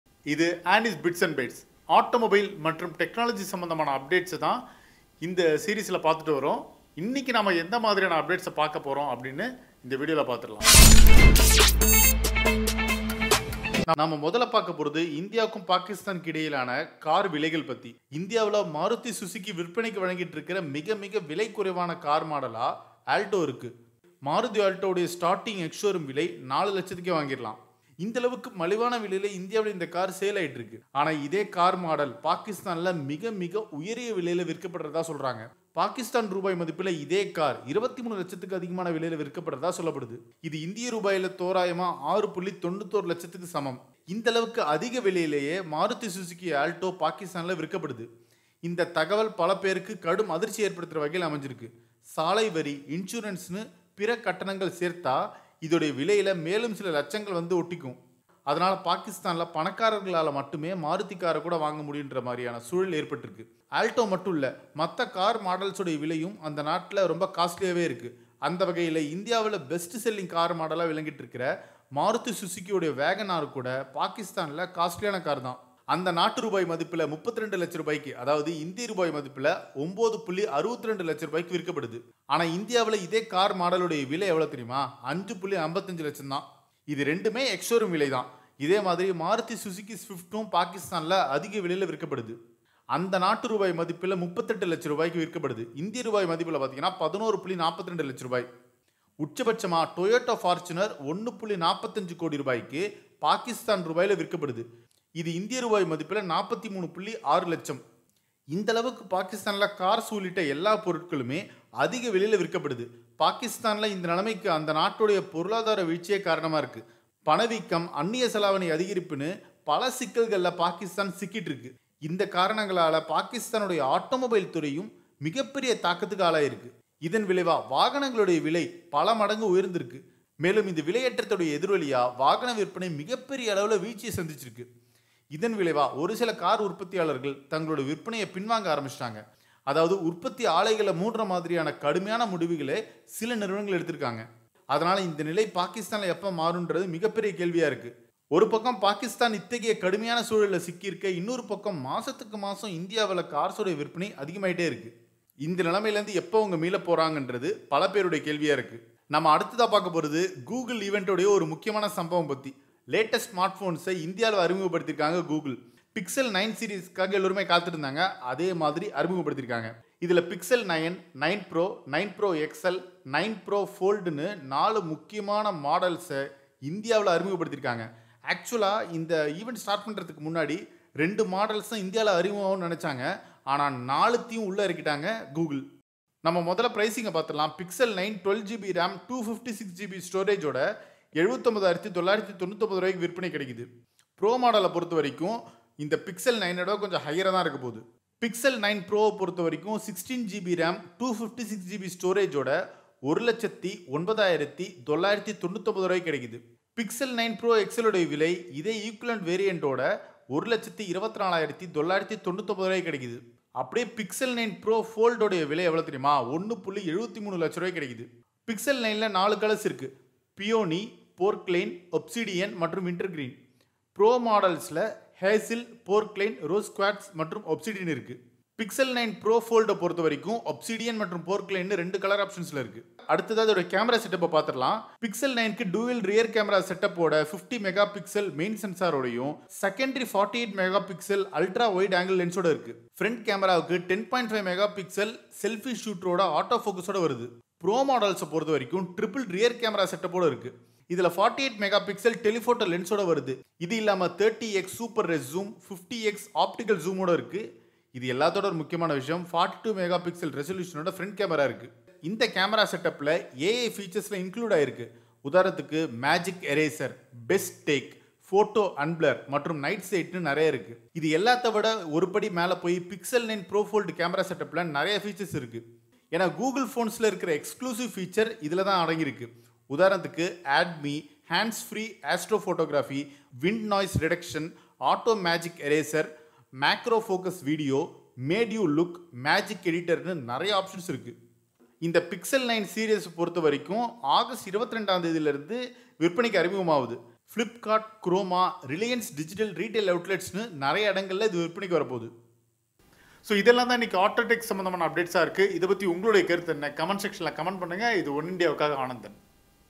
Google Launch செய்த Pixel 9 Series Phones! Ind vs Pak Car Prices` எப்படி இருக்கு? | Oneindia Tamil
Aanee's Bits and Bytes: எதிர்பார்த்தபடியே  Made By Google Event 2024 Pixel 9 Series-ன் கீழ் மொத்தம் 4 ஸ்மார்ட்போன்கள் உலகளவில் அறிமுகம் செய்யப்பட்டுள்ளன; இந்தியாவில்  Maruti Suzuki  விற்பனைக்கு வழங்கிக் கொண்டிருக்கும் மிக மிக விலை குறைவான கார் மாடலாக Alto) இருக்கின்றது.

~PR.55~ED.72~HT.302~CA.37~